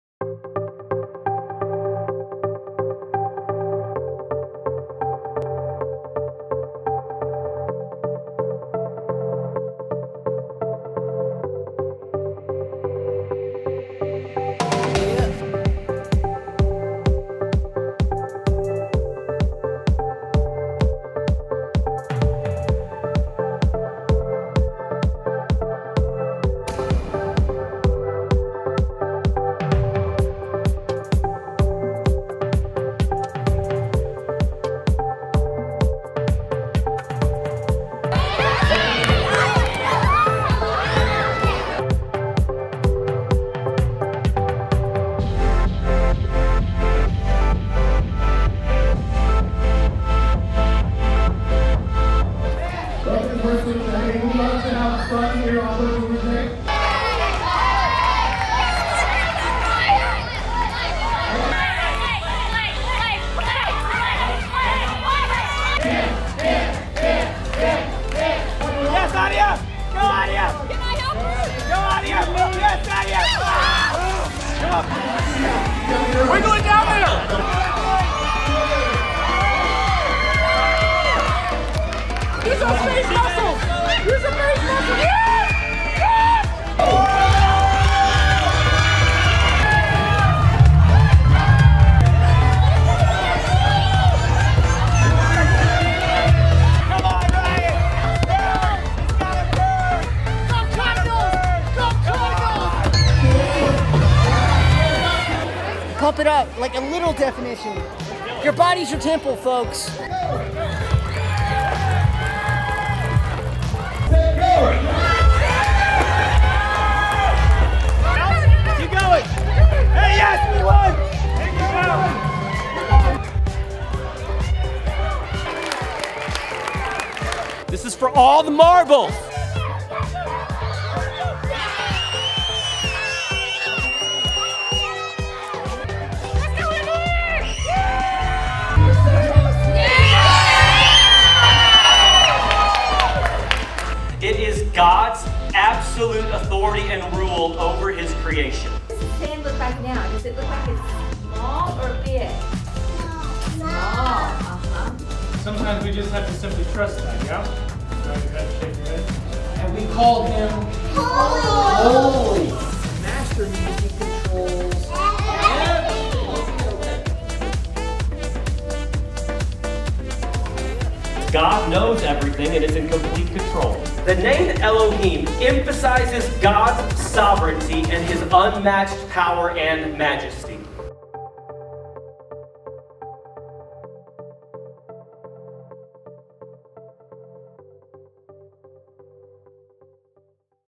First Wednesday October 2019
This is a highlight video of our First Wednesday from The Gathering Baptist Church in Kansas City, Missouri.